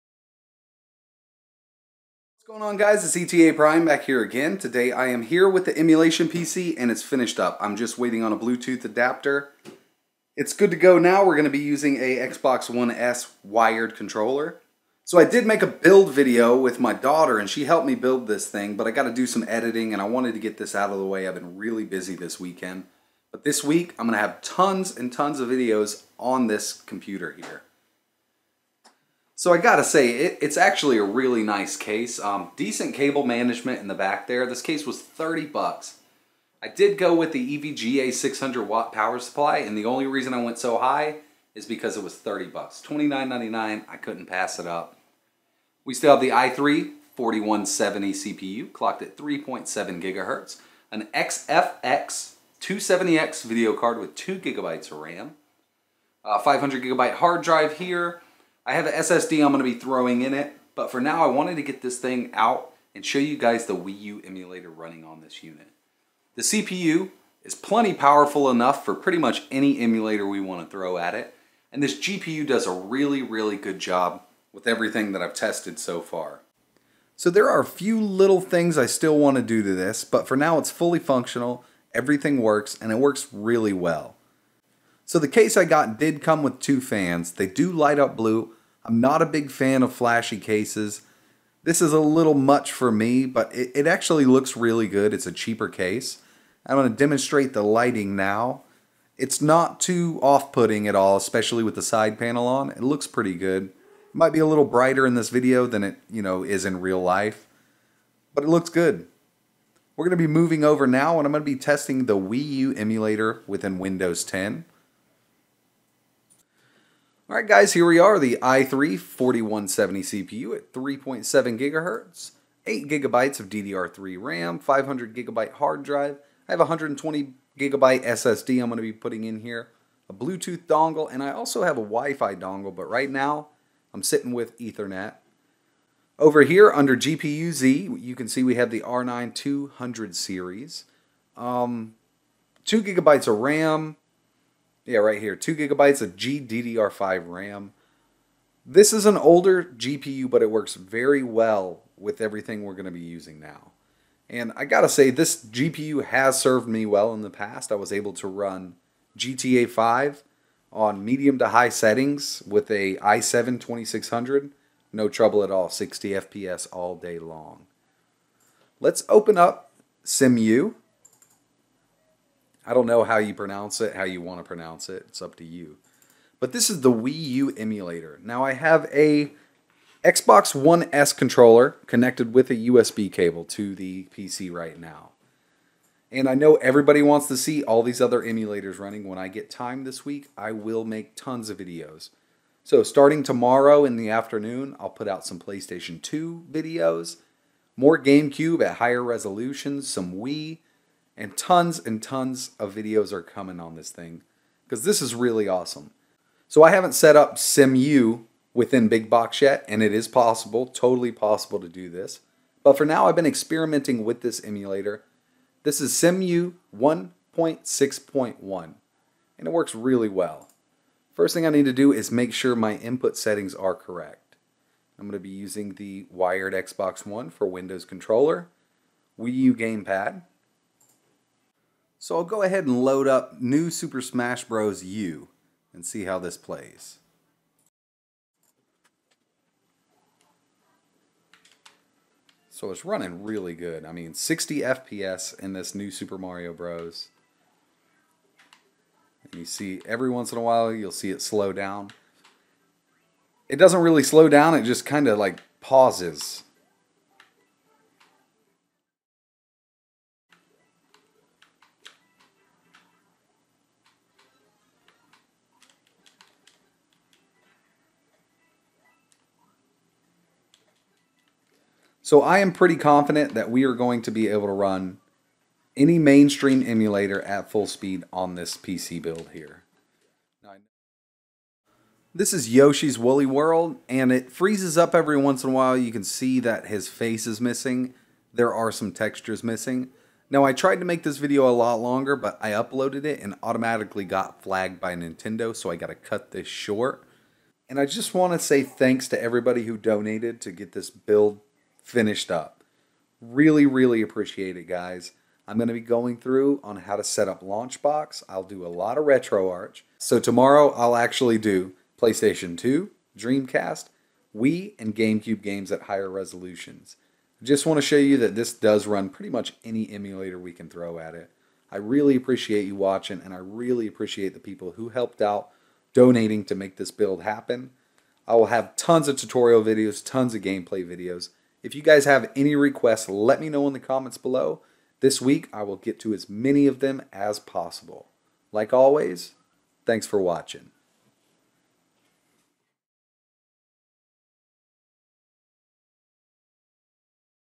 The Emulation PC Build is Done Wii U Emulator Test CEMU 1.6.1
So it's done, guys!! Well Almost just have a few small things to do like add the SSD , Wifi, And Bluetooth.

I JUST WANT TO THANK EVERYONE WHO HELPED OUT WITH THIS BUILD!!! THANK YOU SO MUCH!!

The build video is coming very soon!

PC SPECS
CASE- DIYPC DIY-F2-W
PSU- EVGA 600 Watt
CPU- i3 4170
RAM- Single stick 8gb ddr3 Kingston
Video Card- XFX 270x 2gb
Hard drive - 500GB western digital Blue

Just need to add the SSD , wifi and Bluetooth.